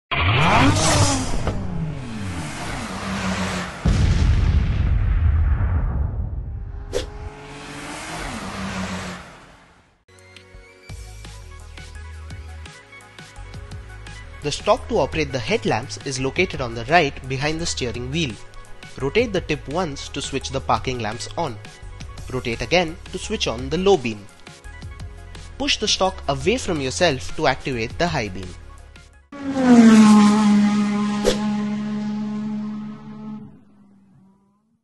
How to Operate Headlamps in Alto K10 | CarDekho.com
How to operate headlamps in Alto 800:
The stock to operate the headlamp is located on the right side behind the steering wheel. Rotate the tip once to switch on the parking lamps. Rotate again to switch on the low beam and push the stock towards yourself to activate the high beam.